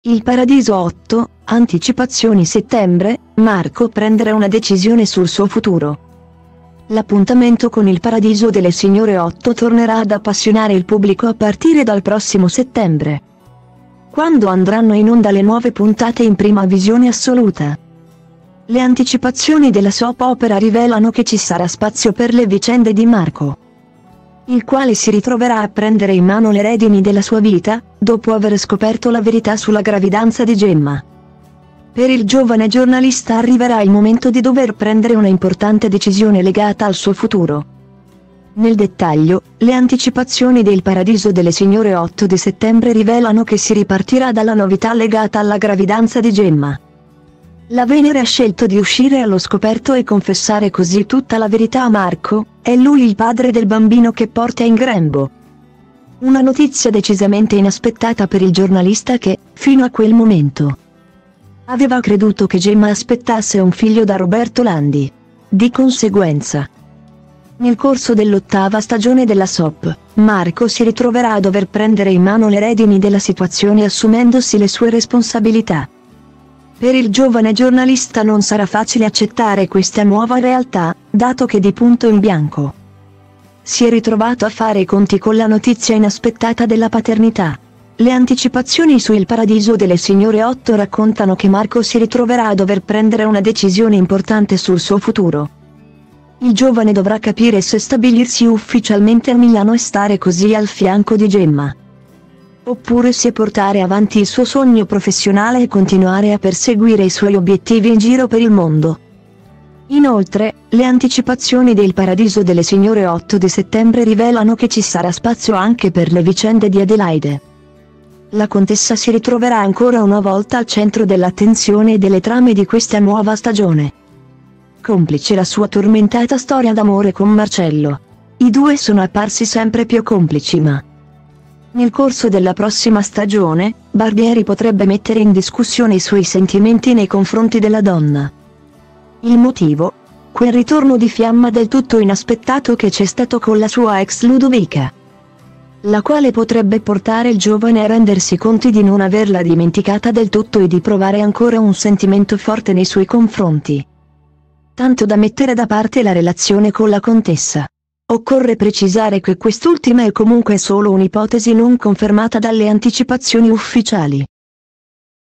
Il Paradiso 8, anticipazioni settembre: Marco prendere una decisione sul suo futuro
L'appuntamento con Il Paradiso delle signore 8 tornerà ad appassionare il pubblico a partire dal prossimo settembre,quando andranno in onda le nuove puntate in prima visione assoluta. Le anticipazioni della soap opera rivelano che ci sarà spazio per le vicende di Marco,